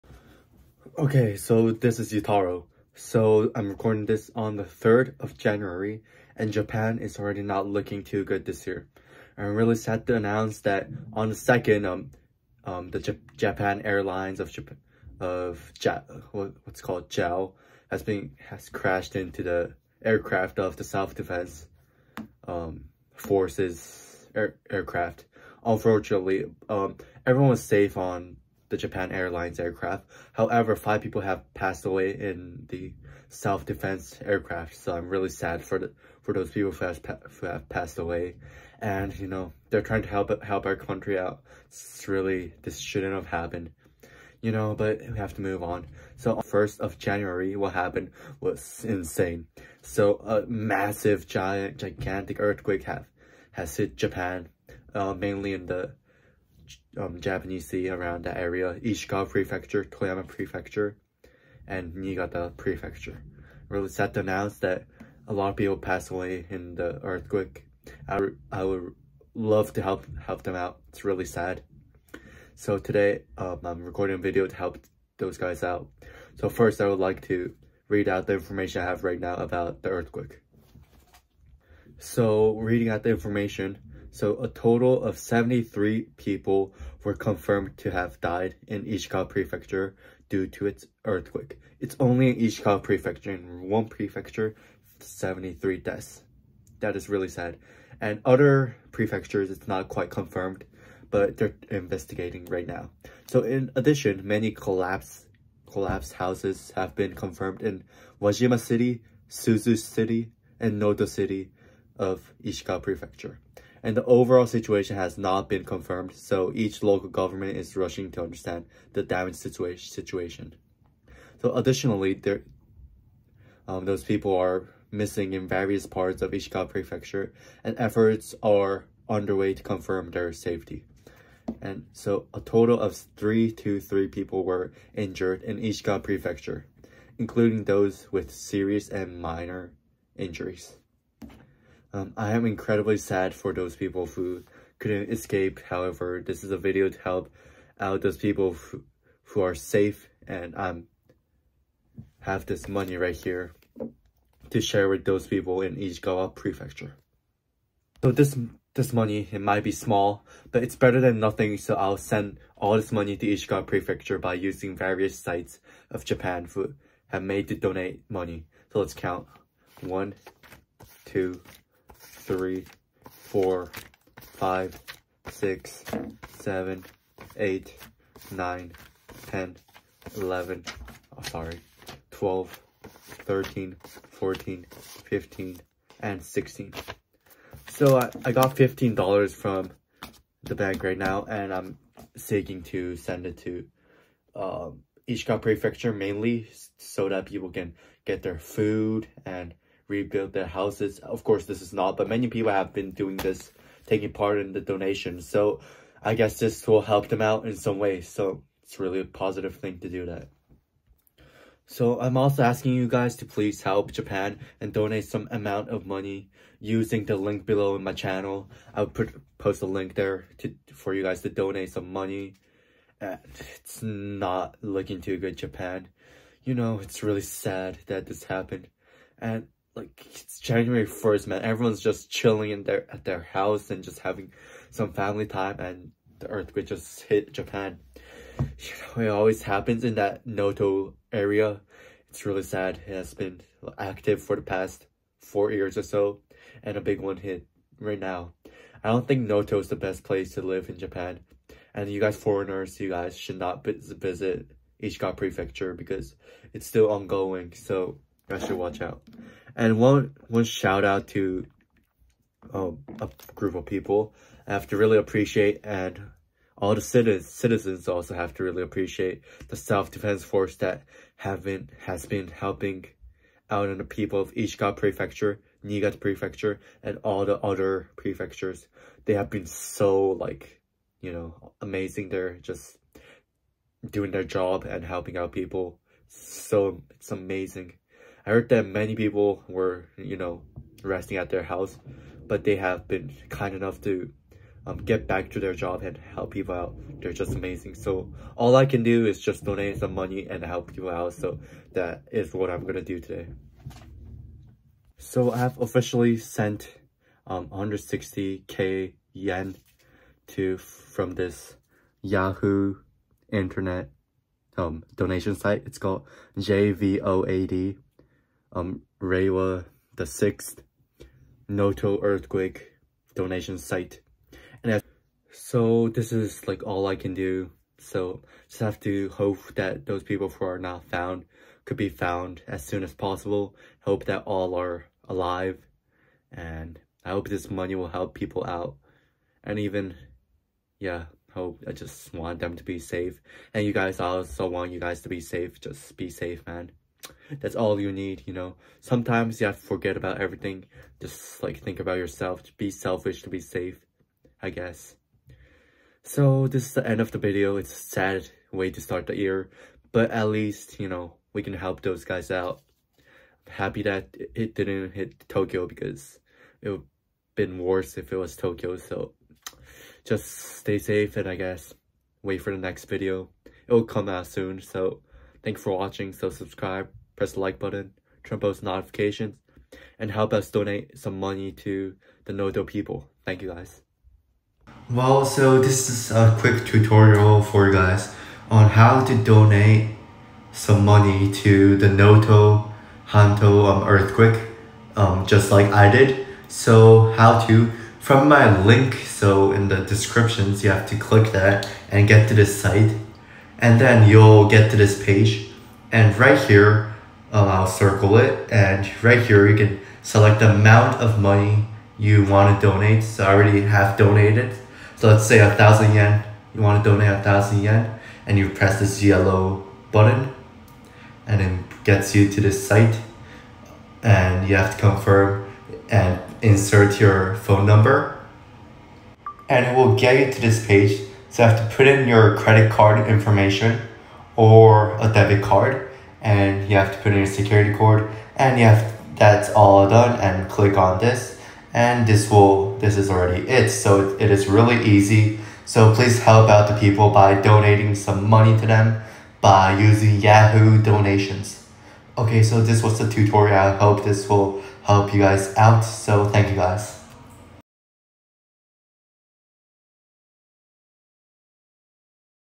[100万円募金します] 能登半島地震 Let’s donate together for the 2024 Noto Peninsula Earthquake?
▼Reliable information
・NHK